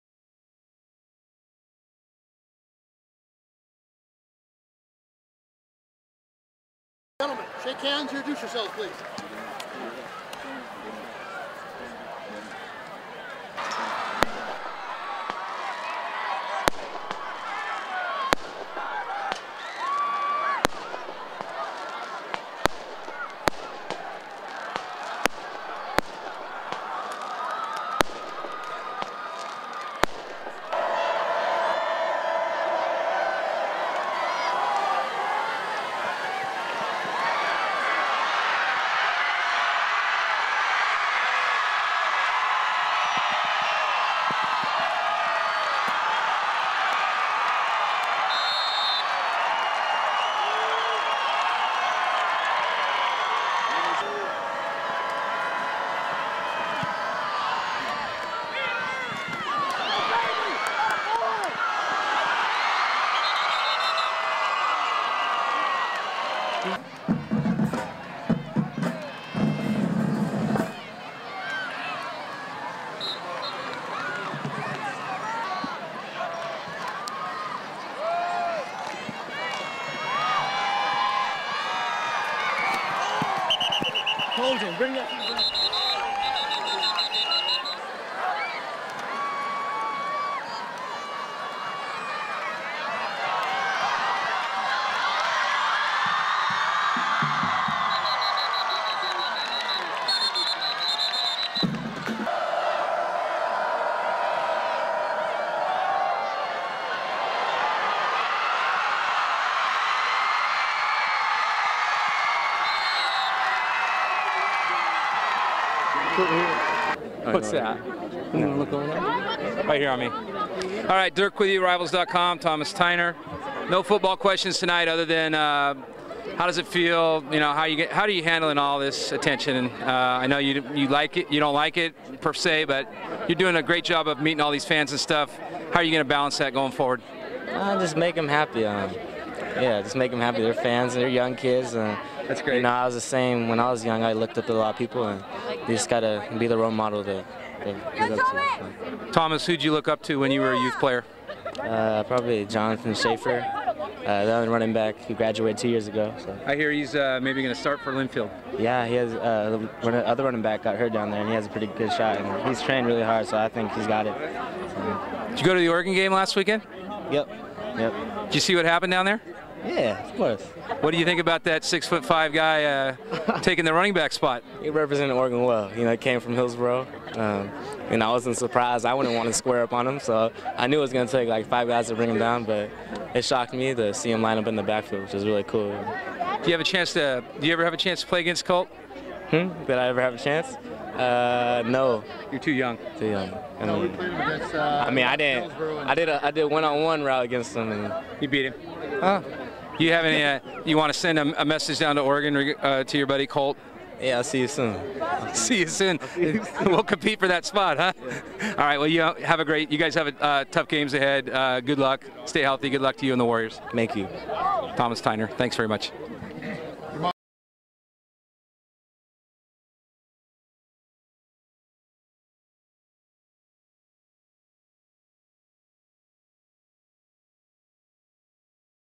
Thomas Tyner vs Westview
Thomas Tyner highlights vs Westview.  Friday September 28th, 2012